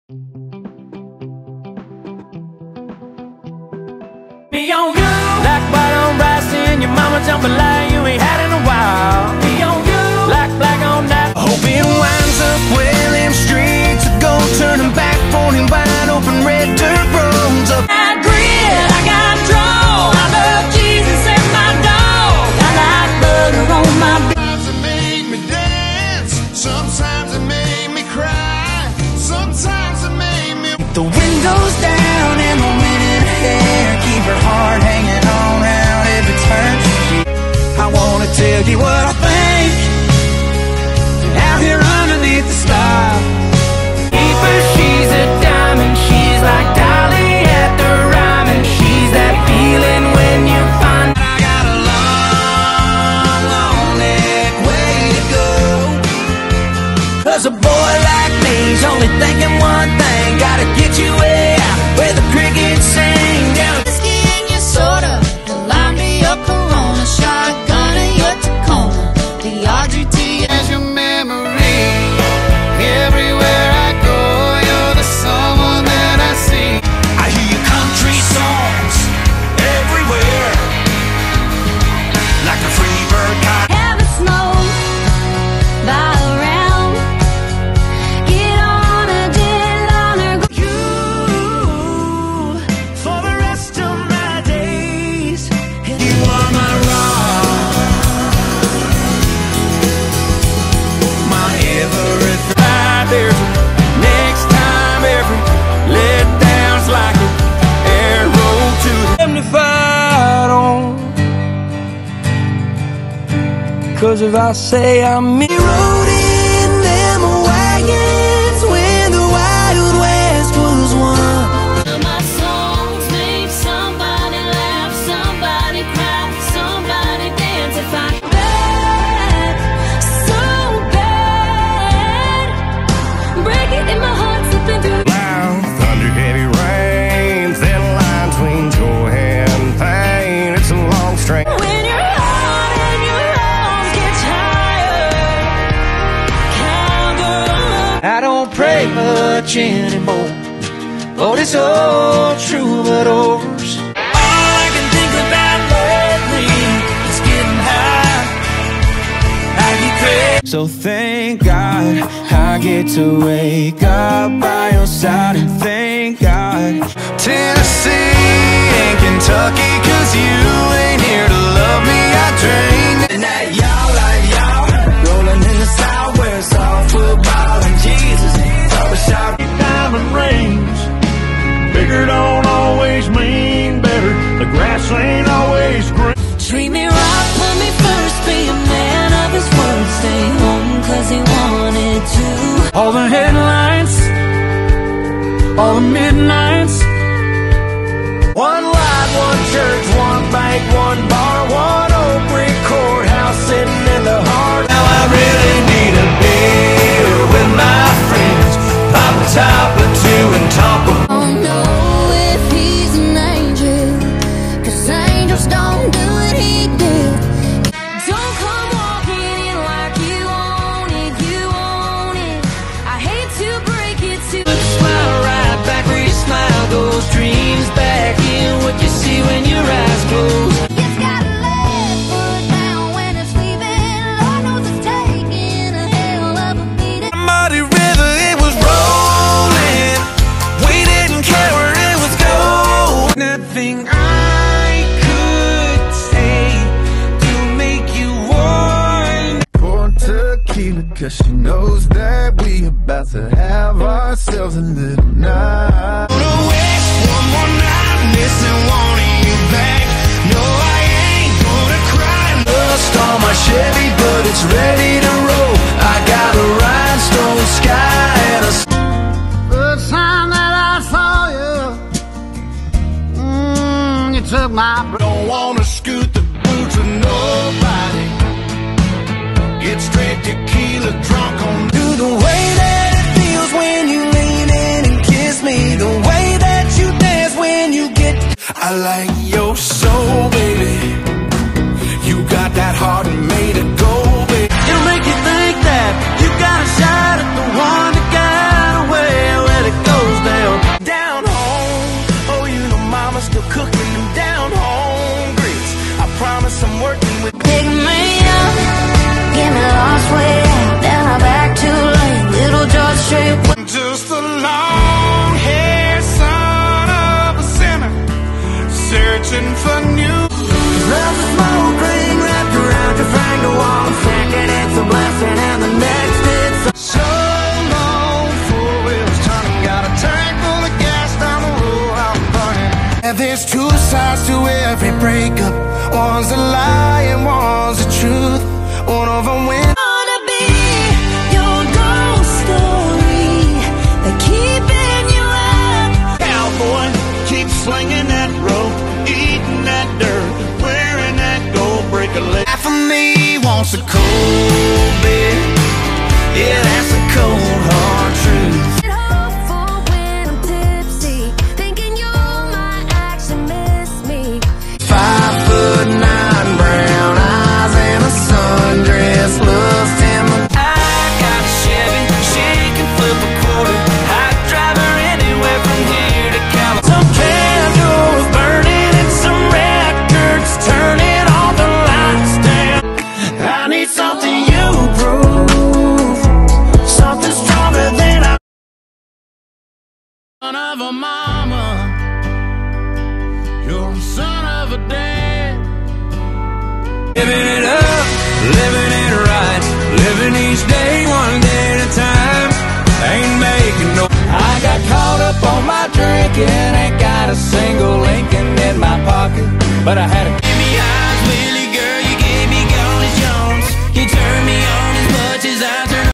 Billboard Top 60 Country Indicator Chart (Oct 1, 2022)
Country airplay chart, data from 109 small-market stations.